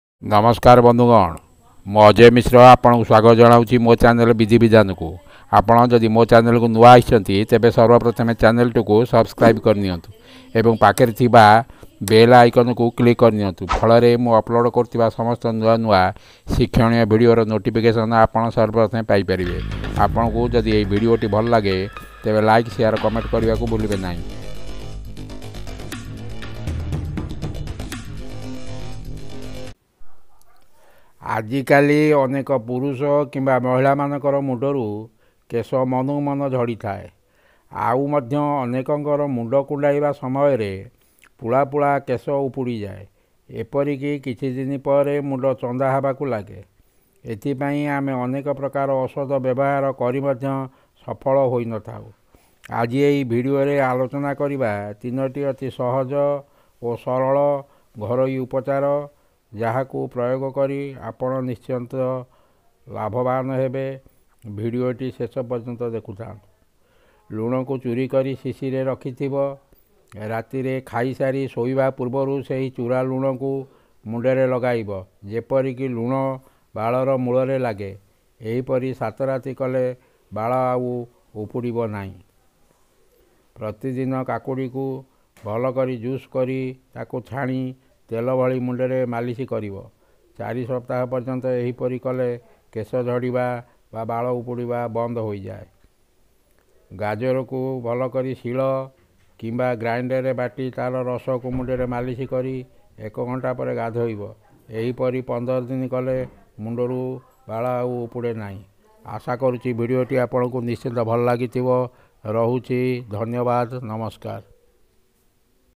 କେଶ ଝଡ଼ିବା ବନ୍ଦ ହୁଏ ।Hair loss is banned ।odia tips ବିଧି ବିଧାନ
hair loss is banned ।Some ayurvedic home treatment for hair loss ।ଓଡ଼ିଆ ବାଳ କେଶ ବନ୍ଦ ହୁଏ#ବିଧି ବିଧାନ